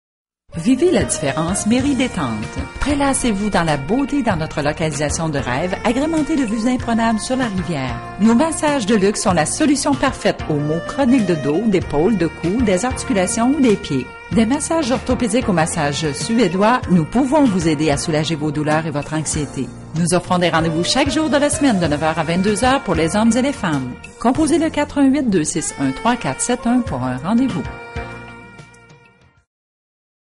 Mérîdétente - Quebec
Mérîdétente
Endroit de rêve, panorama sur le fleuve, massothérapie haut de gamme pour elle et lui. Situé près du musé des beaux art sur la grande allée à Québec nous vous recevons tous les jours de 9hre à 22hre.